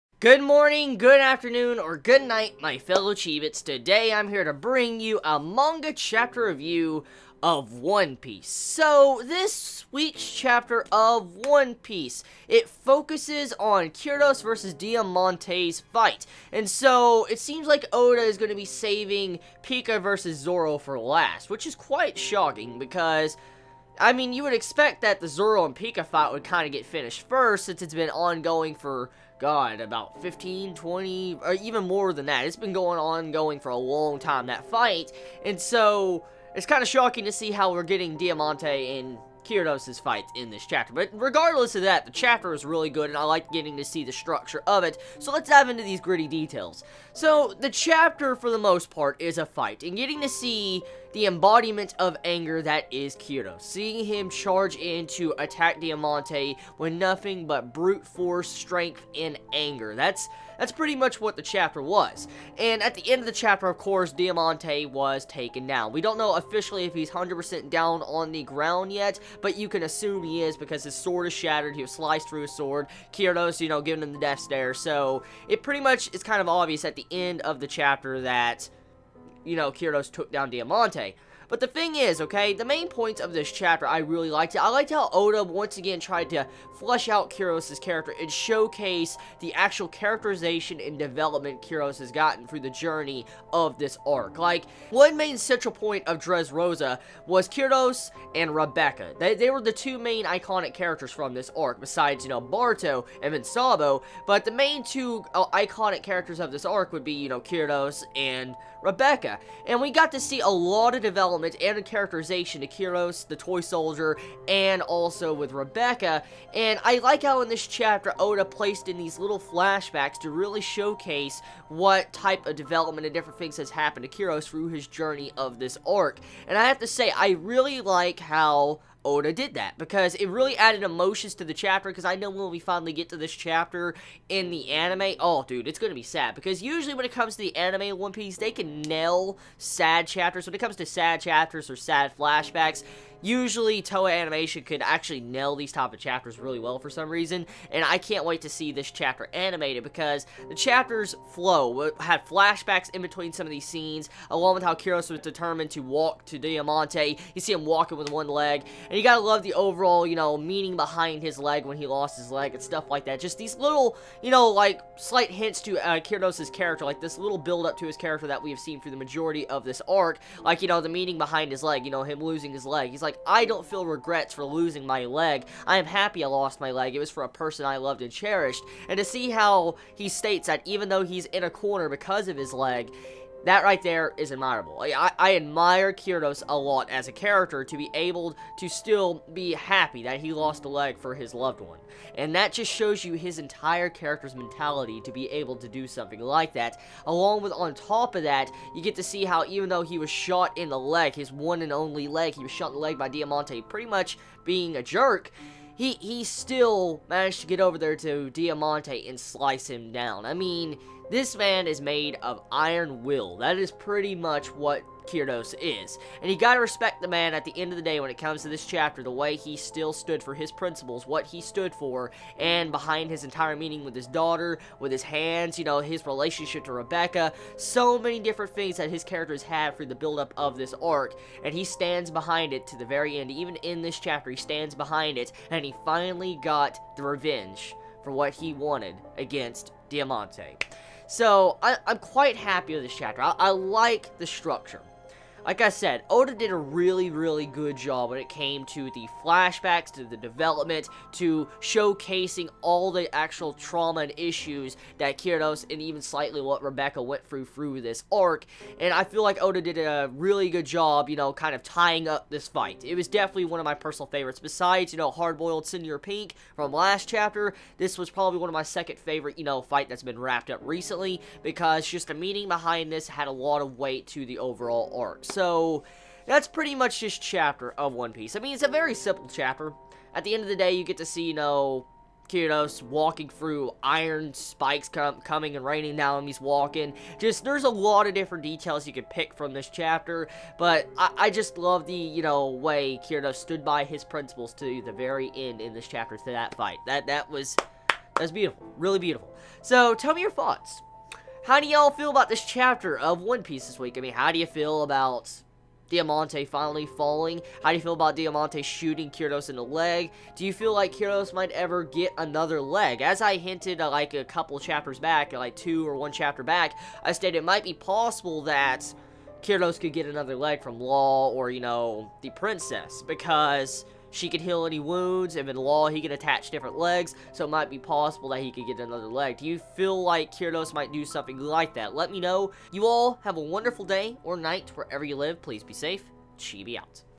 One Piece Chapter 776 ワンピース Manga Review - The Iron Willed Kyros
One Piece Chapter 776 ワンピース showcases the iron willed Kyros vs Diamante. Can't wait to read and review 777.

For any fellow Chibit that wants to send something:
2069 Johnson Chapel Road, 38583, Sparta, TN